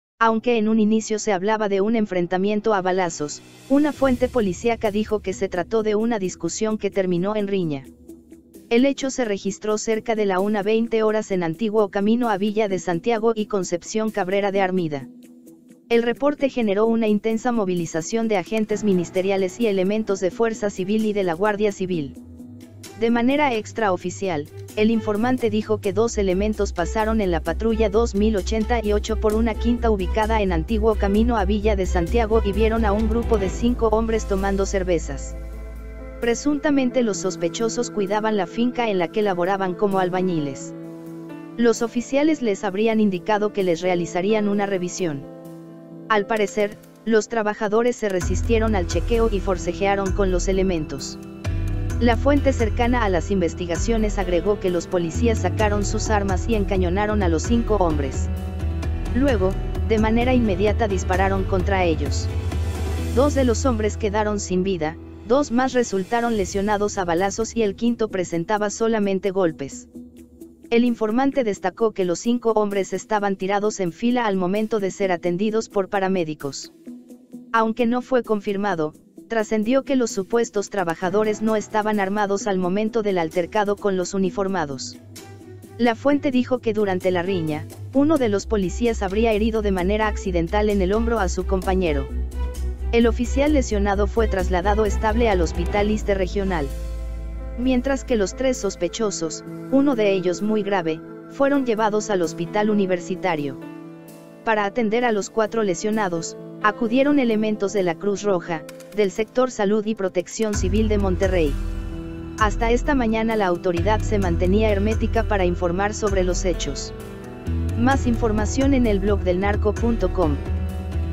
5 albañiles bebían cerveza en una finca que cuidaban, Policías los quisieron someter, se resistieron
Aunque en un inicio se hablaba de un enfrentamiento a balazos, una fuente policiaca dijo que se trató de una discusión que terminó en riña.

5 albañiles bebían cerveza en una finca que cuidaban, Policías los quisieron someter, se resistieron...

El hecho se registró cerca de la 1:20 horas en Antiguo Camino a Villa de Santiago y Concepción Cabrera de Armida.  El reporte generó una intensa movilización de agentes ministeriales y elementos de Fuerza Civil y de la Guardia Civil.  De manera extraoficial, el informante dijo que dos elementos pasaron en la patrulla 2088 por una quinta ubicada en Antiguo Camino a Villa de Santiago y vieron a un grupo de cinco hombres tomando cervezas.